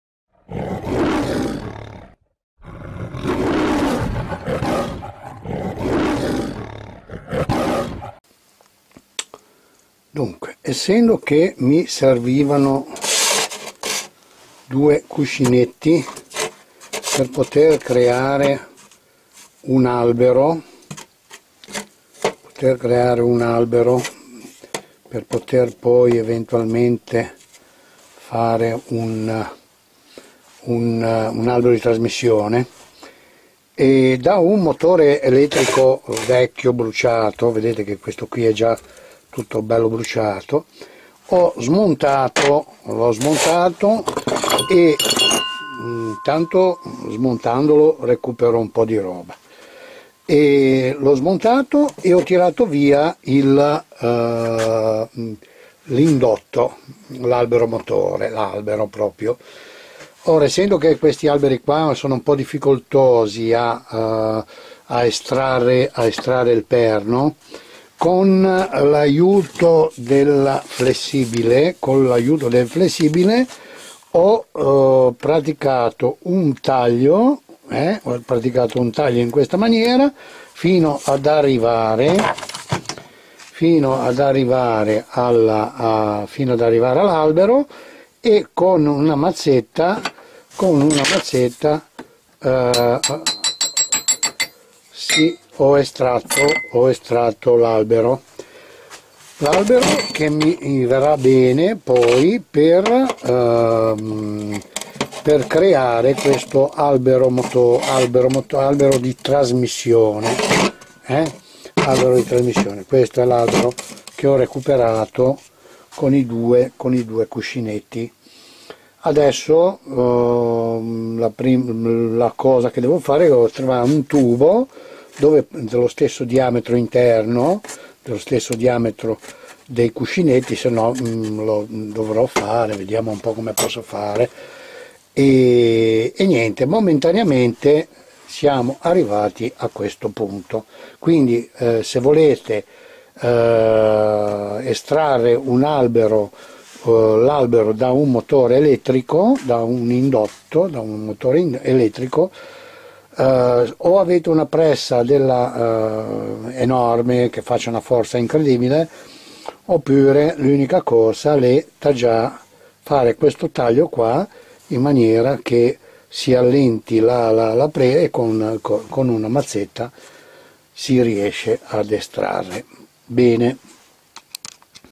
come estrarre l'albero da un'indotto di motore elettrico
nn avendo il tornio e dovendo fare un albero di trasmissione ho utilizzato estraendolo l albero di un indotto di un motore elettrico